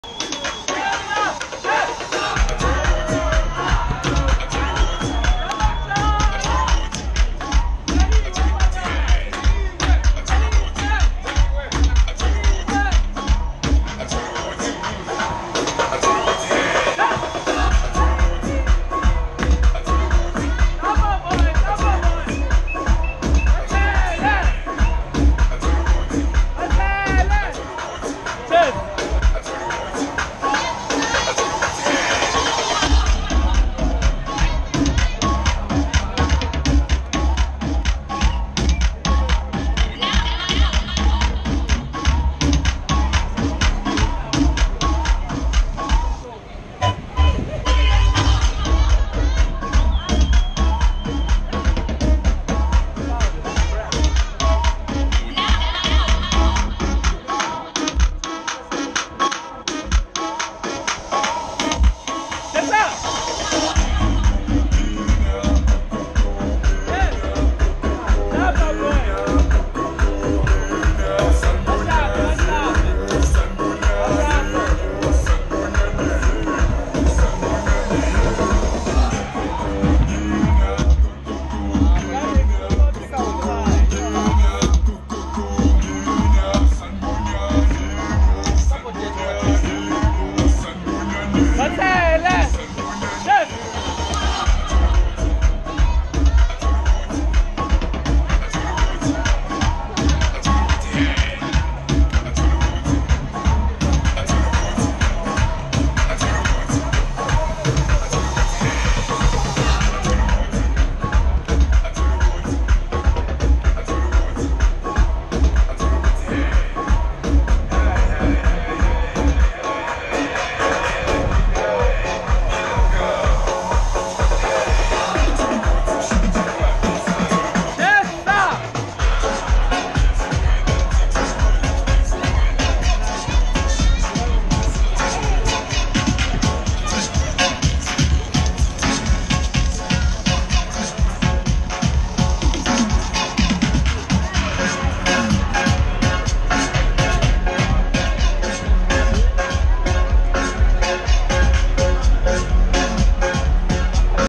Lil DJ With Baby Beats Jumpstarting Kindergarten Disco Party
Worlds Youngest DJ Arch Jnr starting a disco party for his kindergarten friends.